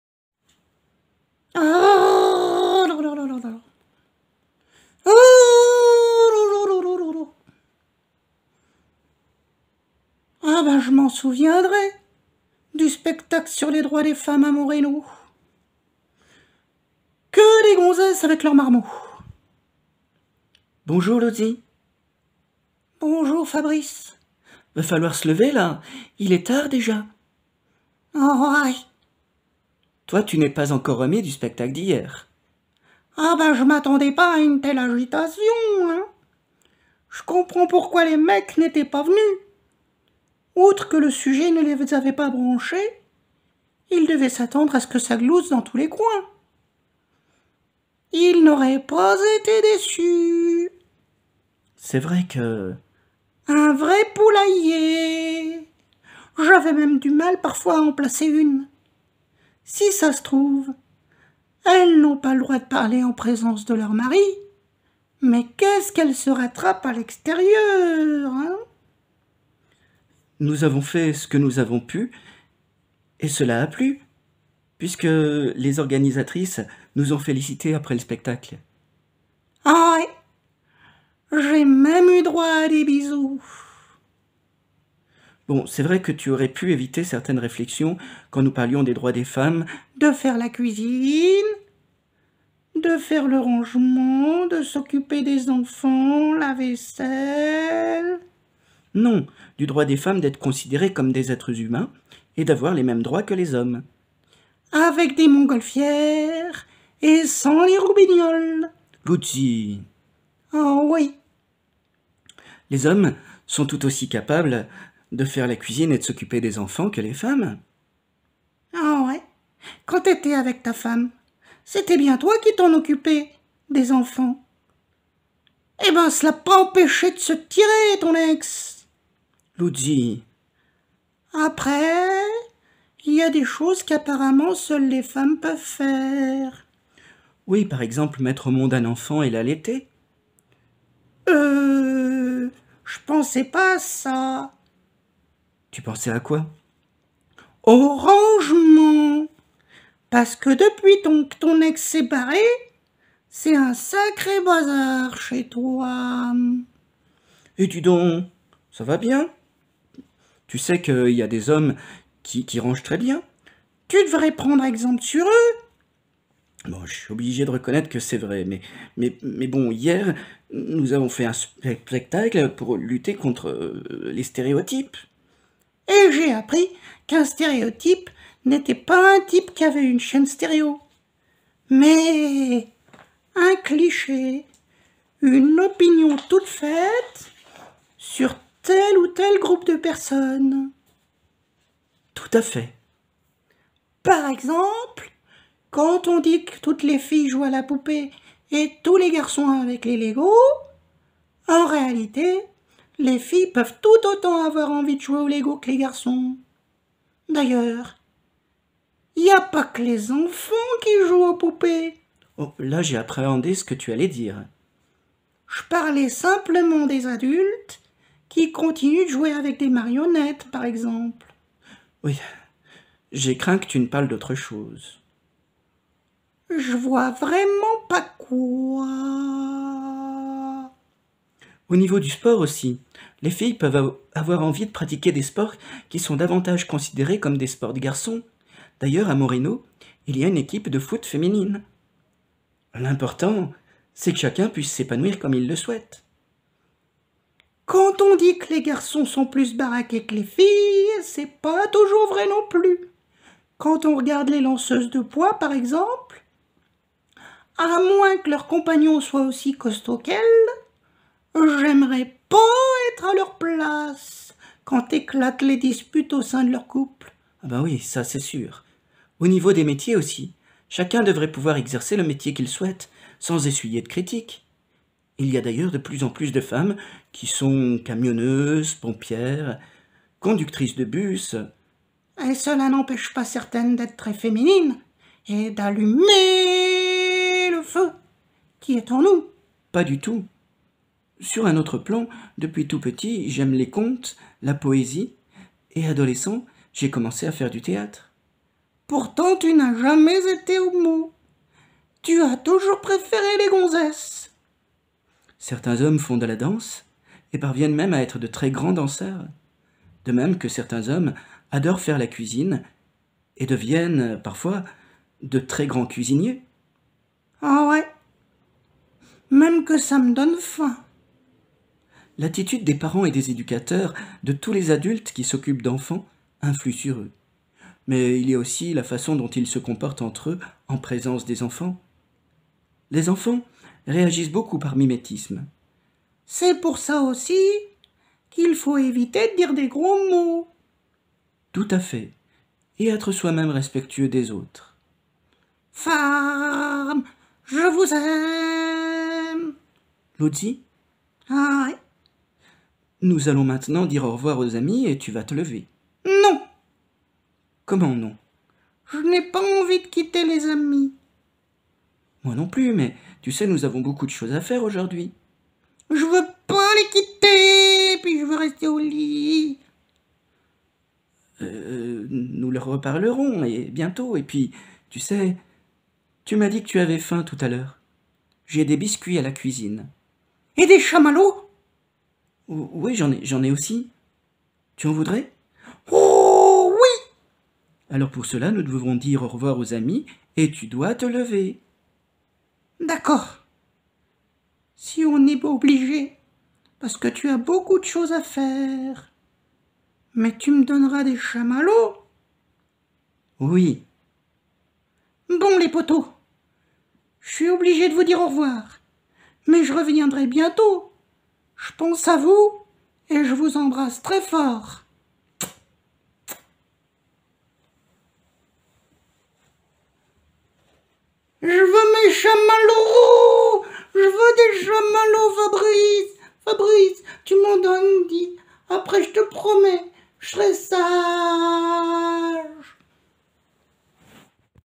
Lutzi et les droits des femmes
Lutzi, notre sympathique  marionnette, a dû mal à se remettre du spectacle qu'il a donné la veille à Montreynaud à Saint Etienne sur "Le droit des femmes" et où il a trouvé des personnes plus bavardes et indisciplinées que lui...